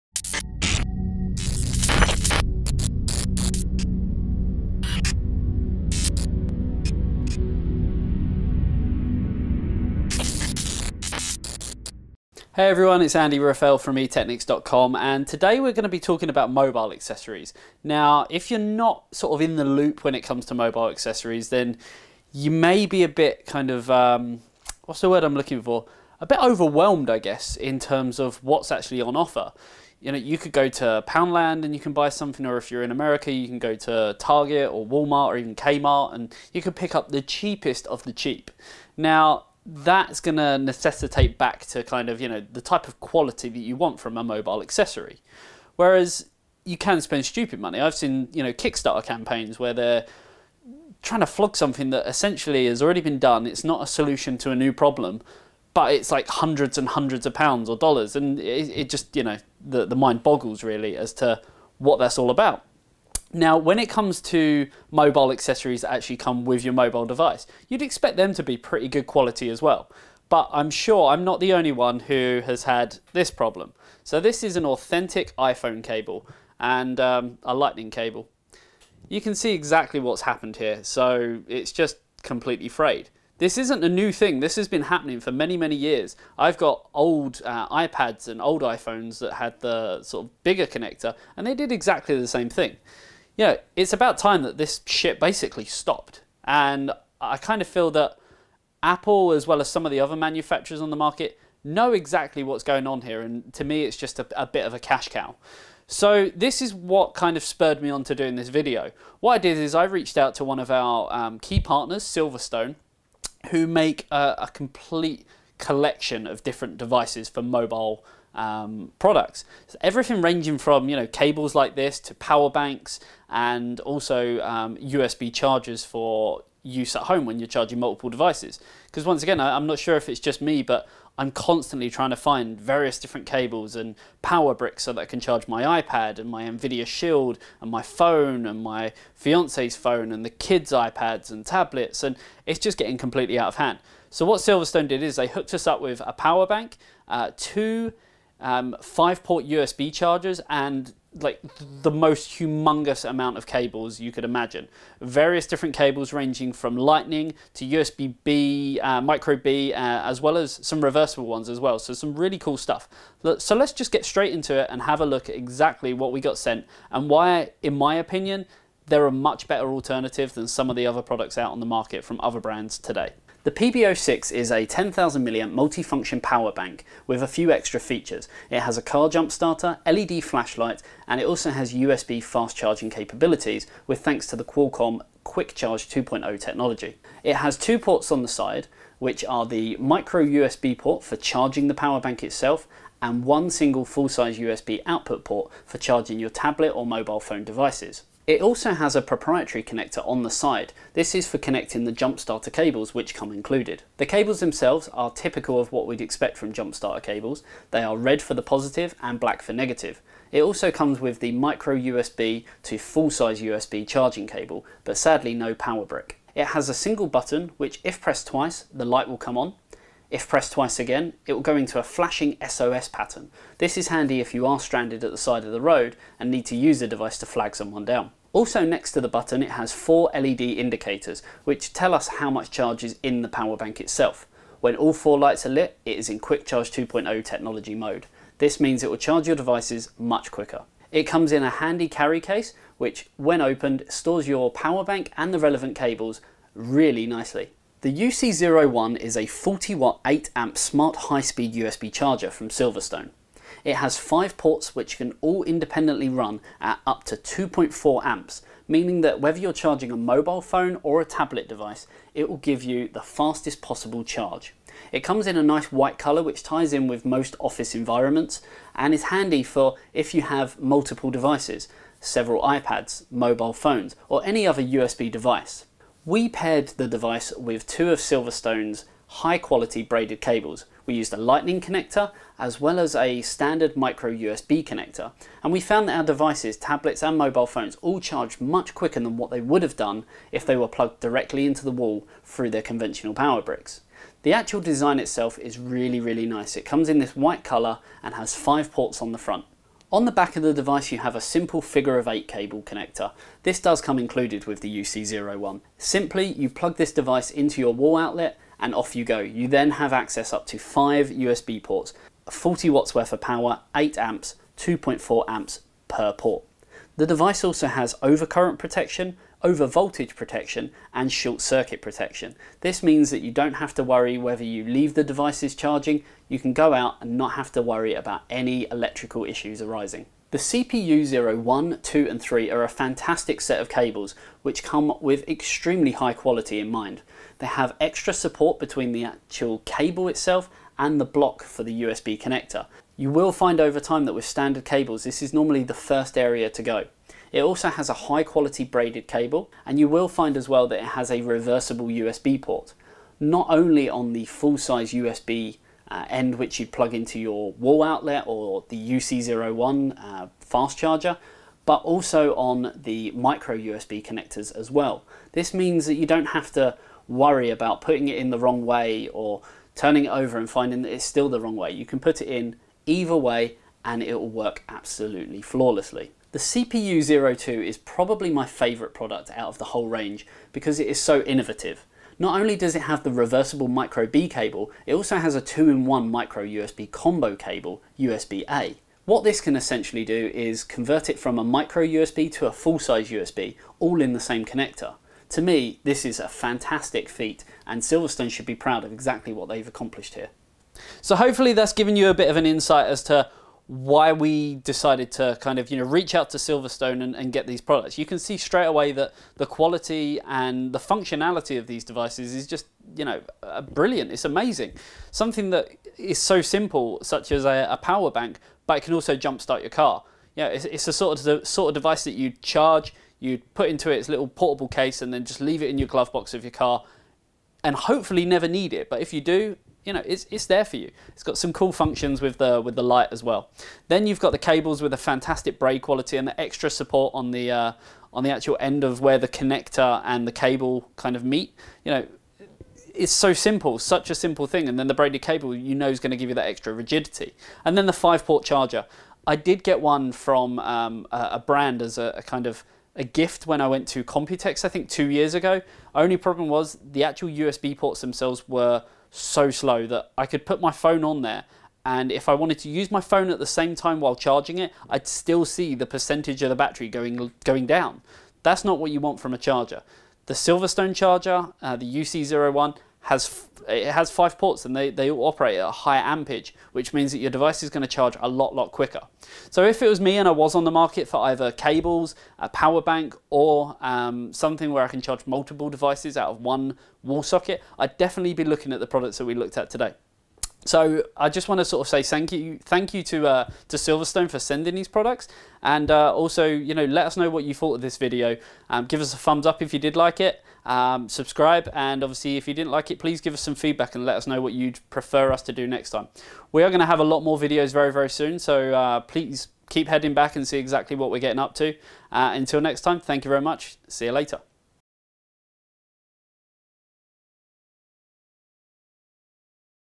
Need Good Quality Mobile Accessories? - Silverstone Power Bank, USB Charger, Braided Cables
After being sick and tired of buying cheap cables and being let down by sub-par mobile devices, we reached out to Silverstone to see if they had anything that would do what we needed, and would last for a long time.

See what they came up with in this video.
►UK Links◄
Silverstone BP06 10000 mAh Multi-Function Power Bank
Silverstone UC01 40W 8A 5 Port High Speed USB Charger
Silverstone CPU01 Reversible USB-A to Reversible Mico-B cable with nylon braided design
Silverstone CPU02 2 in 1 Micro-USB combo USB-A to Reverstible Micro-B cable
Silverstone CPU03 Reversible USB-A to Lightning cable